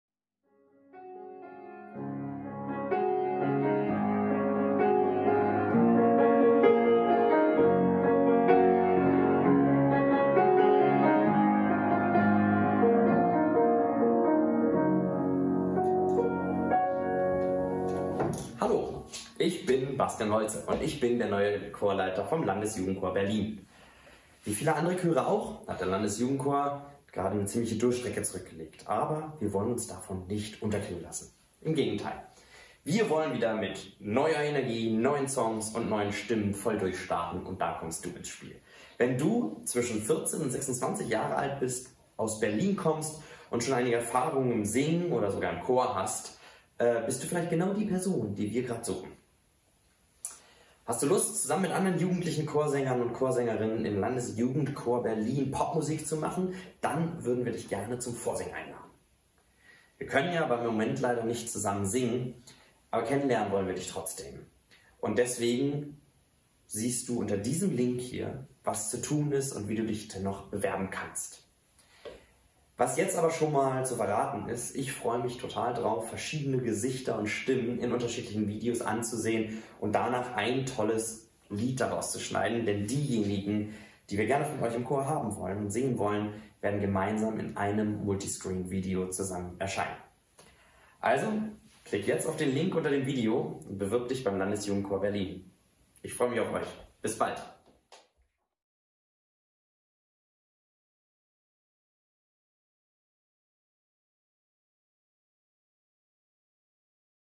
Landesjugendchor Berlin | Sei dabei!
Bewirb Dich jetzt beim Landesjugendchor Berlin

Der Landesjugendchor Berlin hat nicht nur seinen neuen Chorleiter Bastian Holze gefunden, sondern möchte frisch und motiviert mit neuen Mitgliedern ins nächste Jahr starten.
Wie auch Du dabei sein kannst, erklärt Dir Bastian Holze in diesem kurzen Video! Wir freuen uns auf Dich!